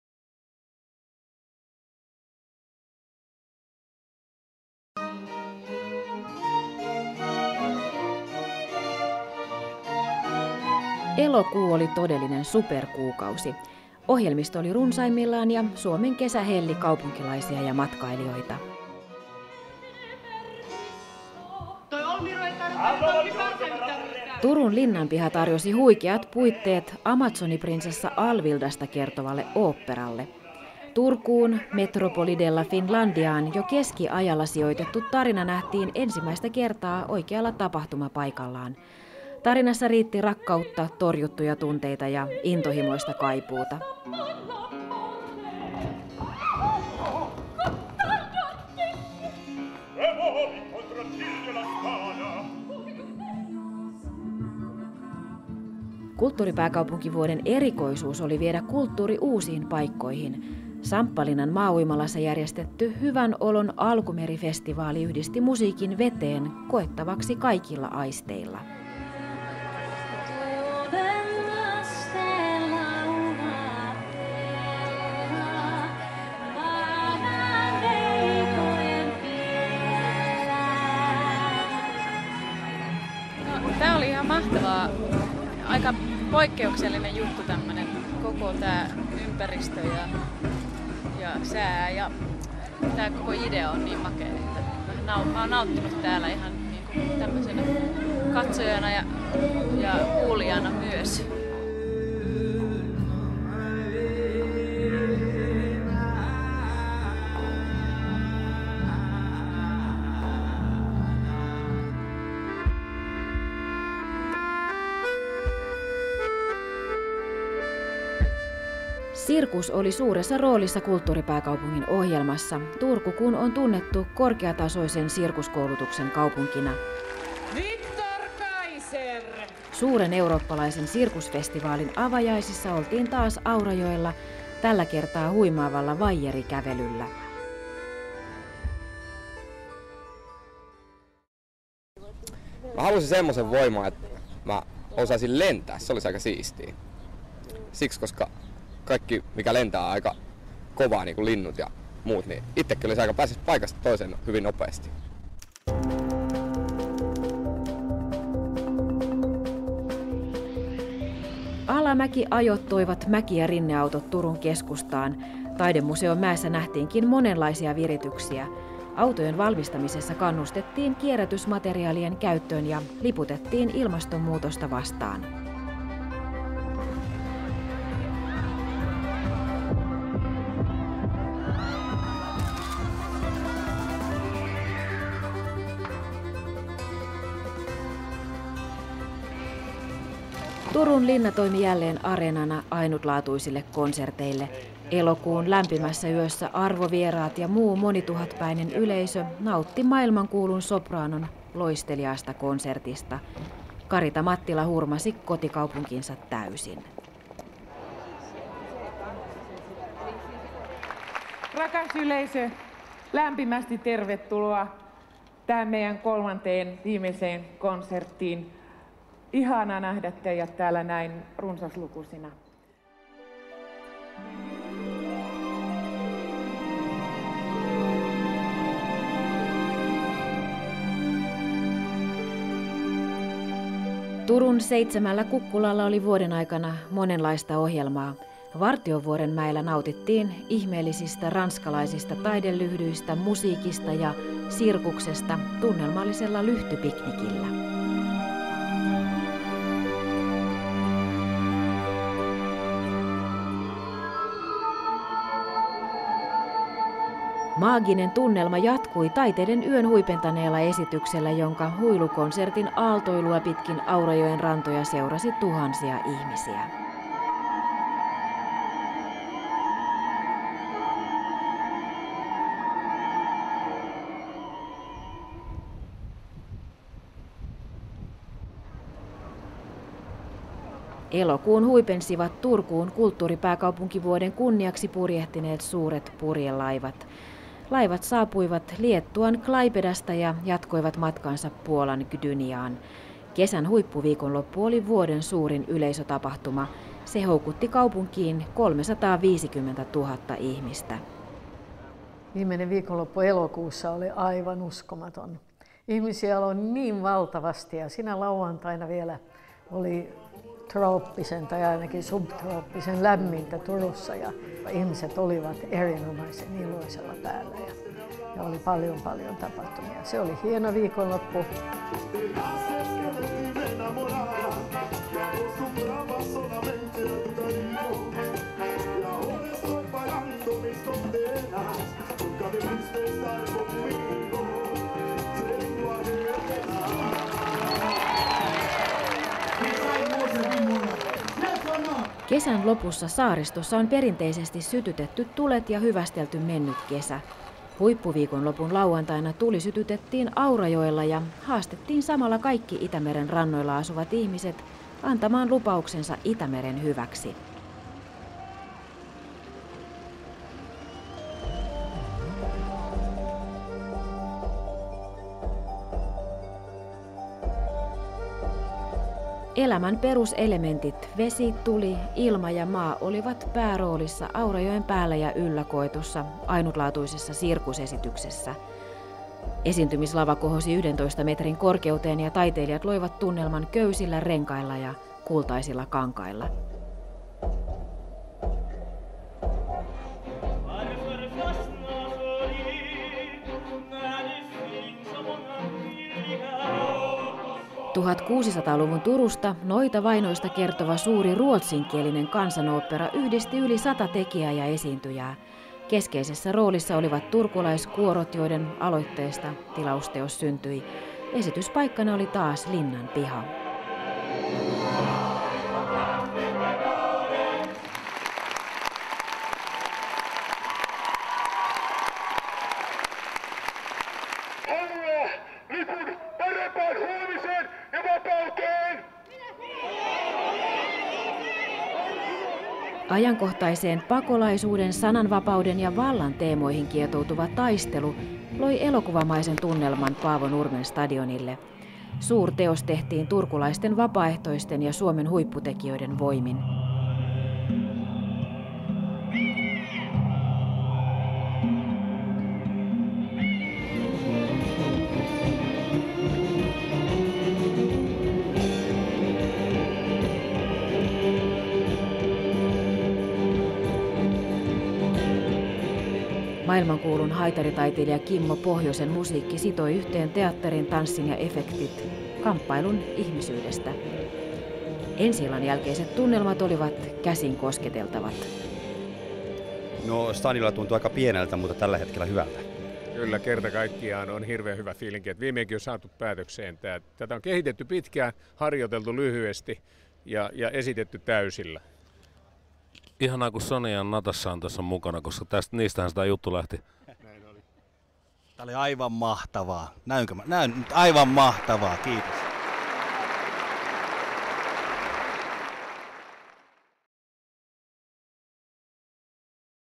Turku's August 2011
Play and Rewind is a dvd-video of Turku's year as the European Capital of Culture in 2011.

Turku European Capital of Culture 2011
Play and Rewind

Written and directed by:
Saara Malila, Turku 2011 Foundation, and Antero Lynne, Focus-Video

Camera operators:
Focus-Video Oy:
Kimmo Impilä
Antero Lynne
Turku Adult Education Centre, Media Work programme
Teachers:
Esa Silander
Kyösti Räsänen
Students:
Kimmo Haapala
Josefiina Honkanen
Juri Jalasmäki
Aki Keskinen
Heidi Kvist
Karoliina Laakso
Teemu Mäkinen
Outi Merilahti
Minna Rinne
Reia Sola
Eero Tahvanainen
Minna Varjonen
Ida Stenholm
Simo Willberg

Turku365 communications:
Ida Hakola
Ilona Hiila
Karoliina Knuuti
Liisa-Mari Ojala
Johanna Salmela
Opening ceremony:
Blue Media Oy

Other camera operators:
Kalle Ahti, Dirtstudio
Kasimir Högnäs, Dirtstudio
Jarmo Röksä, Turku 2011 Foundation

Production: Turku 2011 Foundation and Focus-Video

We would like to thank all those who participated in the making of this film.
Our warmest thanks to the makers and supporters of the Capital of Culture year in Turku.